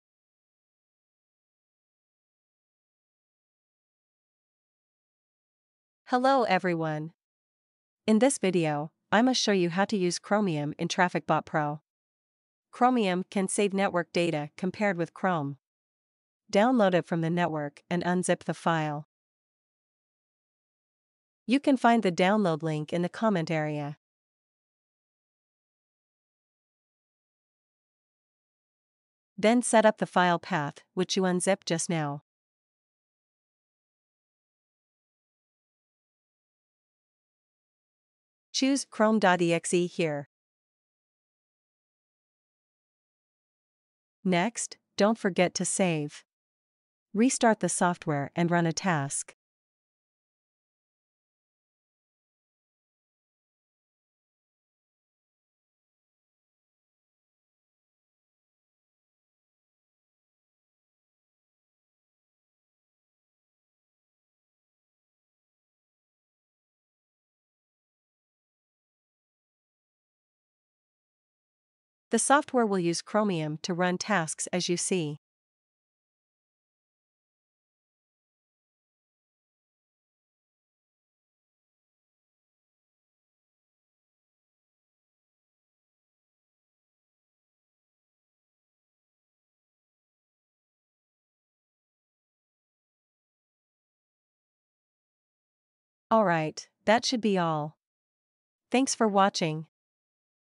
How to use Chromium in TrafficBotPro？
TrafficBotPro is a smart bot which can increase SEO rank, boost traffic and click ads.
It works with Bing, Google, Yahoo, Amazon, eBay, and even more websites.
This video explains how to set up 2captcha API Key to automatically resolve Google Captcha.

If you have any question or requirement, leave a comment to let me know.